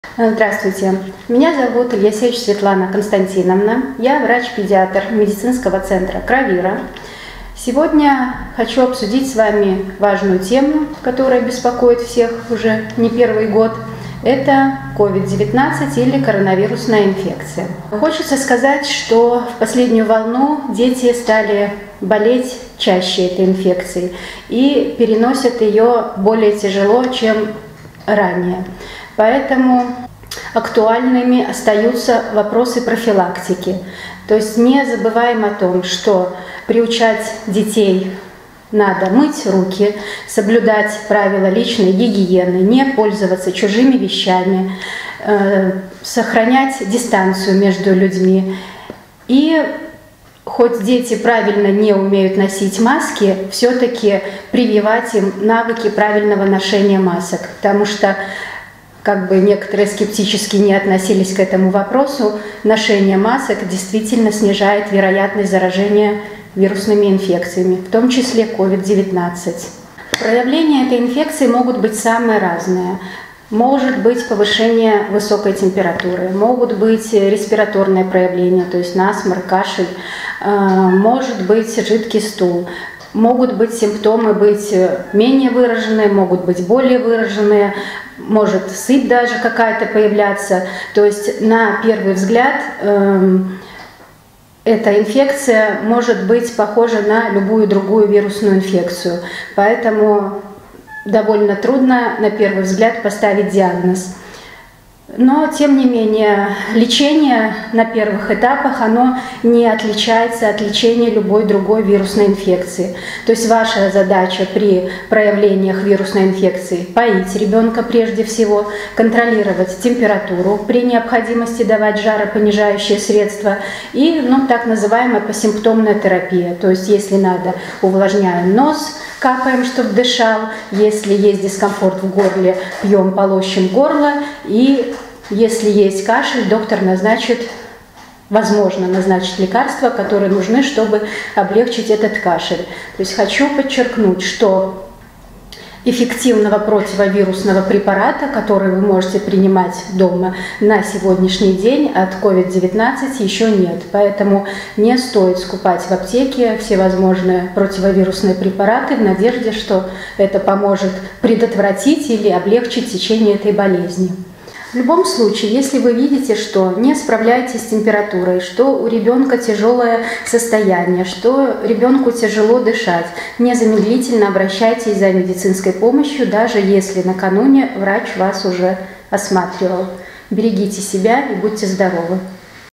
COVID-19 У ДЕТЕЙ
Нужно ли носить маску❓ Какие проявления COVID-19 у детей❓ Как действовать родителям❓ Помогут ли противовирусные препараты (спойлер: нет)❓ Когда нужна незамедлительная помощь❓Ответы на эти вопросы 👇🏻

В нашем новом видео в IGTV! 🍃